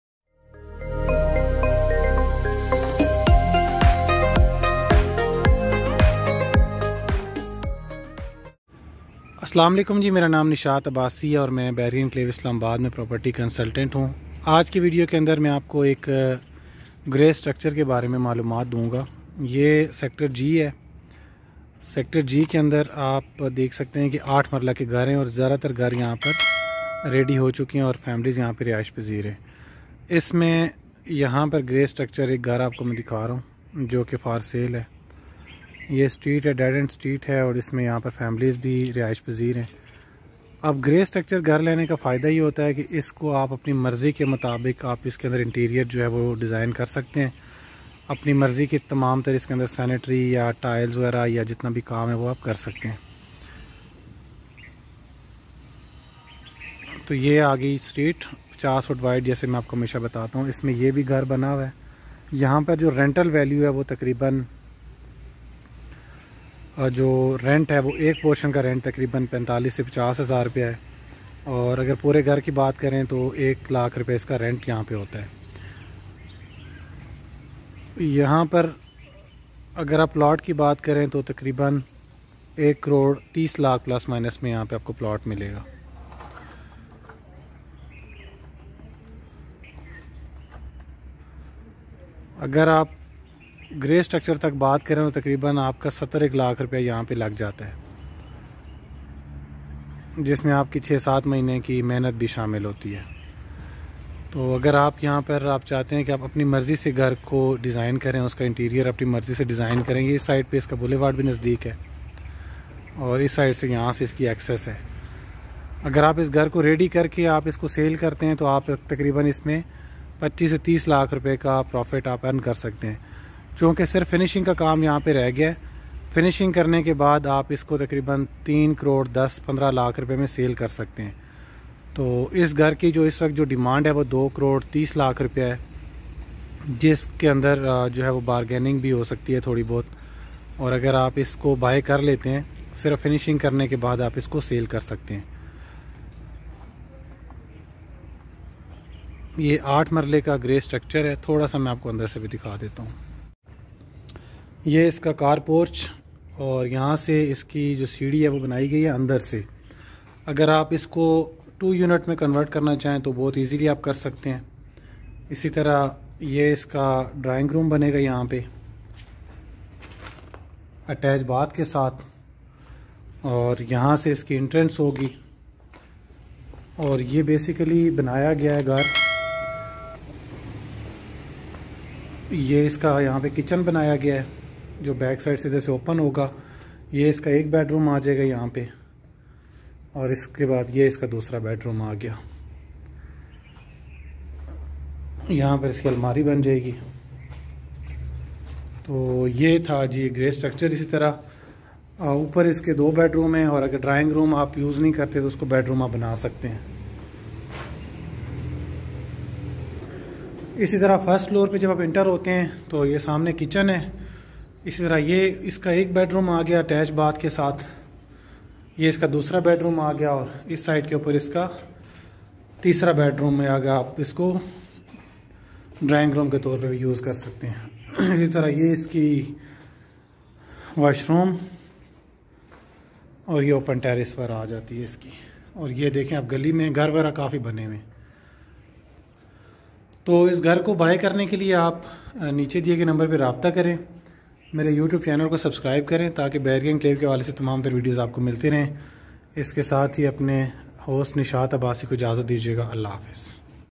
Bahria Enclave Islamabad gray structure 8 Marla House for sale sector G
All facilities available in sector G.
gas water 24/hours electricity
Nayatel PTCL

ideal location
for Details